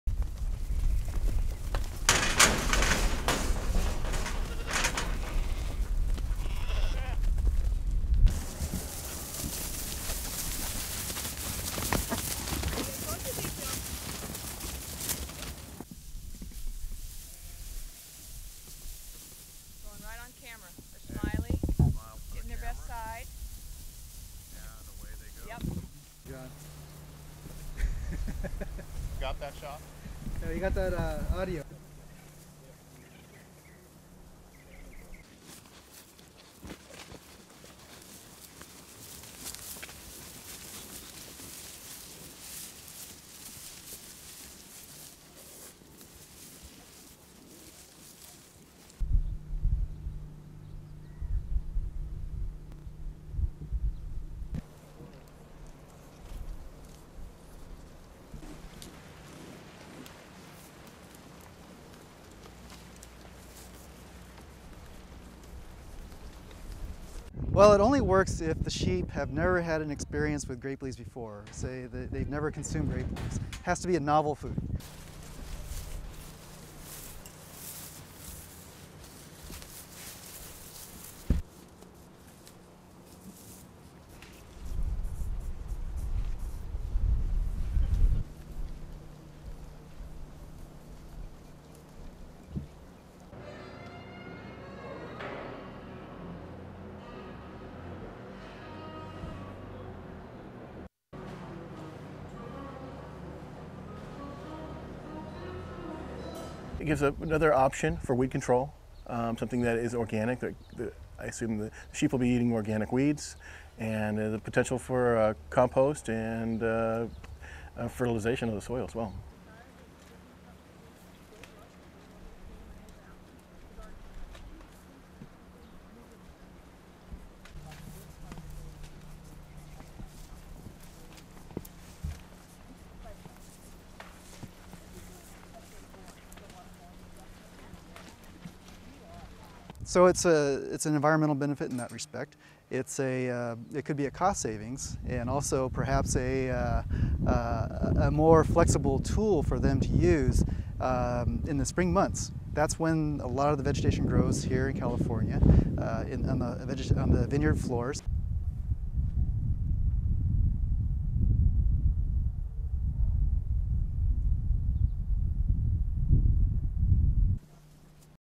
US winemakers give sheep a free lunch
1. Sheep leave pen
2. Sheep rush into vineyard
3. Close-up sheep passing by camera
4. Woman herds sheep
5. Sheep walking through gate
6. Extreme close-up sheep passing by grapes
7. University of California sign
8. Wide sheep grazing in vineyard
9. Close-up sheep grazing in vineyard
10. SOUNDBITE: (English) Morgan Doran, University of California Cooperative Extension:
 "It only works if the sheep have never had any experience with grape leaves before, say they've never consumed grape leaves. Has to be a novel food."
11. Sheep next to bunches of grapes
12. Sheep ignoring grapes
13. Sheep eating weeds
14. Jeriko Winery tasting room with wine bottles in foreground
15. Jeriko wine label
16. SOUNDBITE: (English)  Jeff Cichocki, Wine Producer:
"It gives another option for weed control, something that's organic, I assume the sheep will be eating organic weeds,  and there's potential for compost and fertilisation of the soil as well."
17. Various sheep grazing
18. SOUNDBITE: (English) Morgan Doran, University of California Cooperative Extension:
"So it's an environmental benefit in that respect. It could also have cost savings and be a more flexible tool for them to use in the Spring months. That's when a lot of the vegetation grows here in California on the vineyard floors."
19. "Caution: Sheep on Road" sign
20.  Sheep
LEAD IN:
Wine producers in California are introducing a new environmentally friendly herbicide to protect their grapes.
The new method being tested by the University of California involves inviting sheep into the vineyards and allowing them to feast to their heart's content.
STORYLINE:
It's lunch time for the sheep in Hopland, California.  These eating machines are far from sheepish when it comes to their food. They'll devour just about plant in their path.
So it seems an unusual that researchers at the University of California are asking wine producers to let them loose in their vineyards.
They say there's no such thing as a free lunch. And it's also the case here. The grapes on the menu are part of an aversion therapy training project to teach animals to graze on weeds in the vineyard rather than the grapes.
Morgan Doran from the University of California Cooperative Extension says the new method will only work with sheep that have never eaten grape leaves before.
At first researchers allow the sheep to eat their fill of grape leaves. They then administer a harmless dose of medicine to the animals that makes them feel nauseous.
The sheep blame the grape leaves for the nausea and quickly learn to stay away the next time and stick to the weeds.
Known as mutton mowers by researchers it is hoped the sheep will give wine makers an alternative to herbicides and petrol guzzling tractors.
Wine maker, Jeff Cichocki, says the new technique is an organic option for weed control, and potential for compost and fertilisation of the soil.
Some wine producers are already using small sheep that cannot reach the vines to clear their vineyards of weeds.
But regular sized sheep are cheaper.
Morgan Doran from the University of California Cooperative Extension, says the method is beneficial to the environment and would cut costs for producers.
It's clearly a case of sour grapes for the sheep, but next time you order Californian wine you might be be able to thank a "mutton mower" for your glass of Chardonnay.
Keyword-animals
wacky